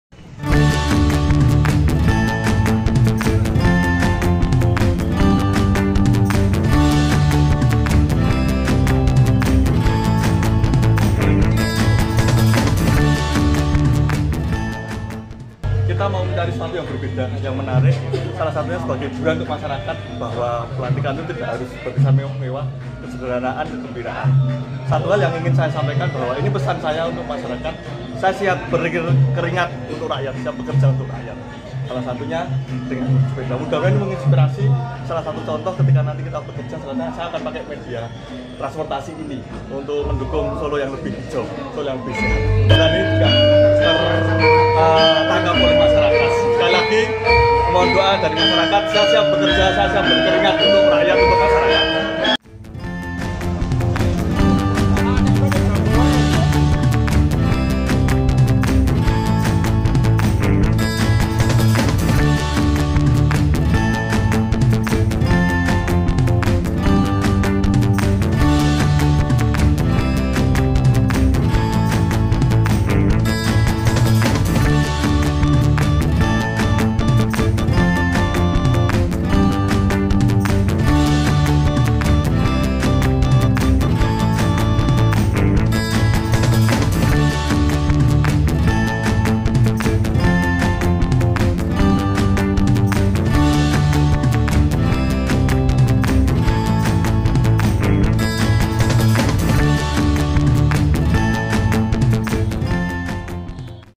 HADIRI PELANTIKAN, MANTAN SATPAM yang Jadi Anggota Dewan Ini "NGONTEL" !!! LIHAT YG TERJADI ...
Solo - Wajah bahagia dan semringah terlihat di wajah Antonius Yogo Prabowo yang akan menghadiri acara pelantikan DPRD Kota Solo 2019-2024.
Ia merupakan satu-satunya calon legislatif (caleg) Partai Solidaritas Indonesia (PSI) yang berhasil lolos menjadi anggota dewan dari Dapil Kecamatan Jebres.
Sebagai wujud syukur lolosnya menjadi wakil rakyat Kota Solo, Yogo pun memilih cara unik untuk berangkat menuju Kantor DPRD Kota Solo untuk pelantikannya sebagai anggota dewan. Ia memilih naik sepeda ontel sebagai tunggangannya menuju lokasi pelantikan.
Yogo tak sendiri naik sepeda ontel, pasalnya sejumlah pendukung dan kader PSI ikut mengawalnya dengan naik ontel. Untuk menuju Gedung DPRD Kota Solo, mereka harus mengayuh ontel sejauh 7 kilometer dari kediamannya di Mojosongo.
Baginya jarak yang jauh dengan ngontel tidak masalah, karena ia ingin menunjukkan kepada masyarakat bahwa dirinya sebagai anggota dewan nantinya siap untuk berjuang dan mengabdi kepada rakyat. Oleh sebab itu, sejak hari pertama sebagai angota dewan ingin menunjukkan sikap yang sederhana dan bekerja keras.